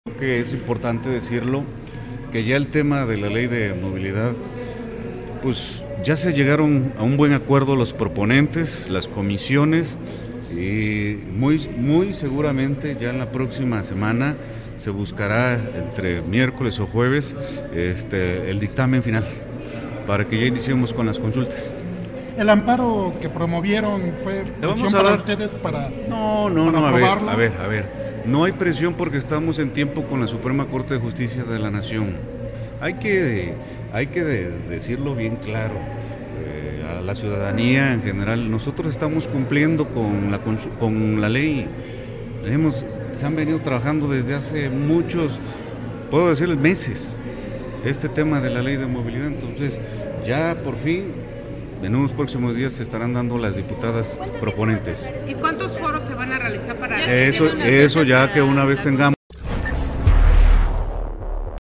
La próxima semana será aprobado el dictamen para iniciar con las consultas sobre la Ley de Movilidad
🧿Será la próxima semana cuando la Junta de Gobierno y Comisiones apruebe el dictamen para iniciar con las consultas ciudadanas sobre la Ley de Movilidad y Seguridad Vial, confirmó el presidente del Congreso de Puebla Eduardo Castillo López

📹Alejandro Camacho